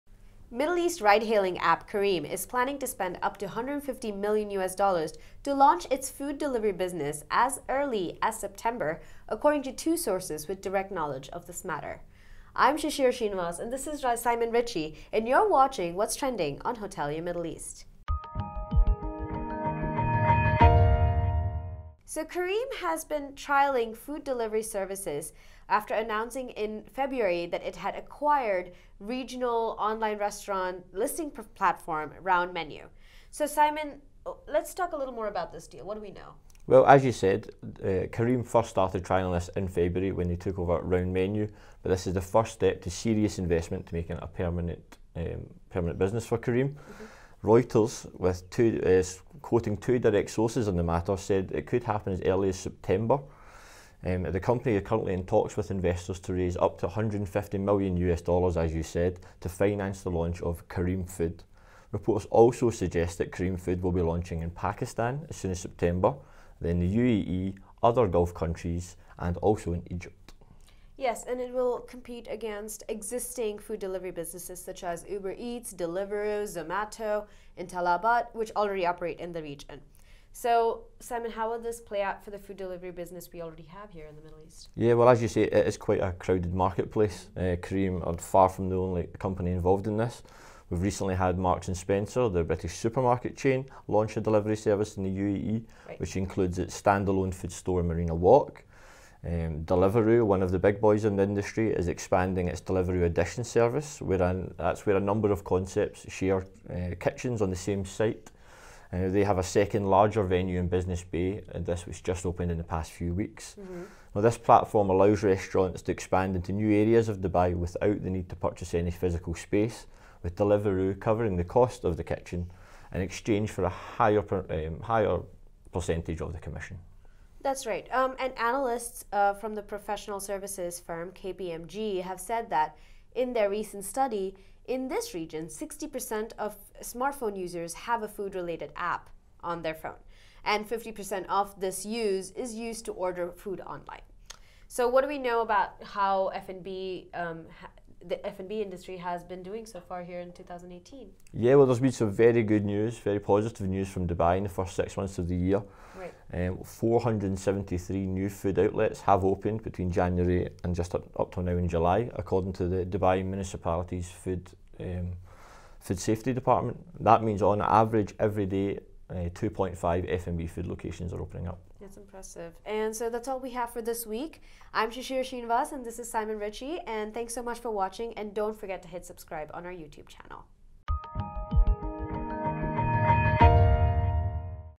How Dubai's food delivery business is changing
Dubai-based Careem is set to take on the likes of Deliveroo and Uber Eats with the launch of its own food delivery service. Reuters has heard from two sources within the ride-hailing company who said Careem is trying to raise up to USD150 million to launch the service, CareemFood, with it potentially coming into action by September. Careem has been undergoing trials since February, when they purchased RoundMenu, using their fleet of drivers to not only ferry passengers but also food orders, in much the same was as Uber does with its Uber Eats delivery service. We have a look at the food delivery business in the emirate of Dubai in this Week's What's Trending on Hotelier Middle East.